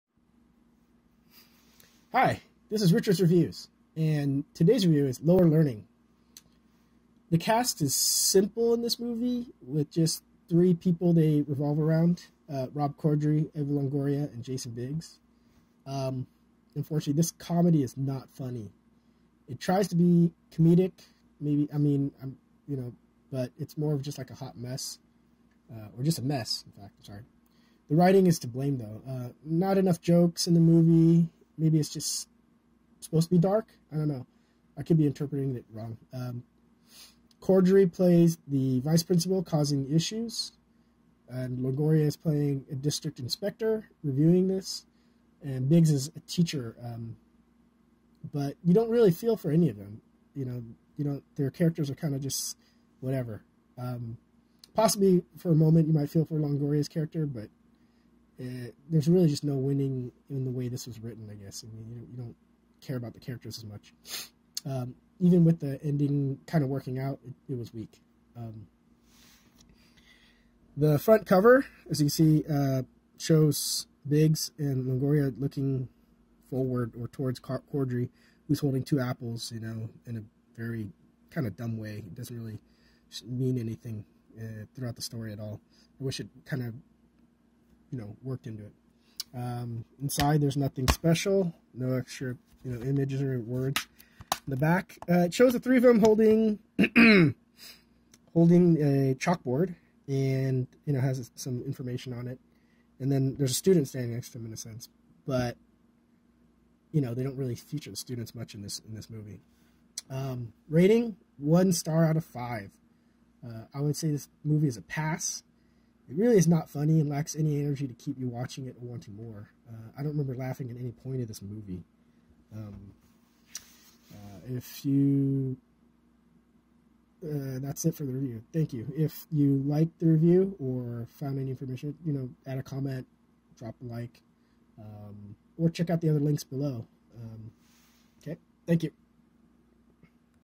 Movie Review 30 - Lower Learning - Video Blog
Hi, this is Richard’s Reviews.

Todays review is Lower Learning. The cast is simple with just 3 people they revolve around. Rob Corddry, Eva Longoria, and Jason Biggs. This comedy is not that funny. It tries to be a comedy, but ends up a hot mess. The writing is to blame. Not enough jokes in the movies. Maybe it is just supposed to be dark. I could be interpreting it wrong. Corddry is the Vice Principal causing issues, and Longoria is the district inspector. Biggs plays a teacher, but you don't really feel for any of them. Possibly Longoria's character for just a bit, but it seems like there is no winning in this story. Even with the happy ending it seemed kind of weak.

The front cover of the box is just the three of them. Nothing special on the inside. The back shows them holding a chalkboard next to a student. They do not really feature the students much.

Rating - 1 Stars out of 5

I would say this movie is a pass. It really is not funny, and lacks any energy to keep you watching or wanting more. I don't remember laughing at any point in this movie.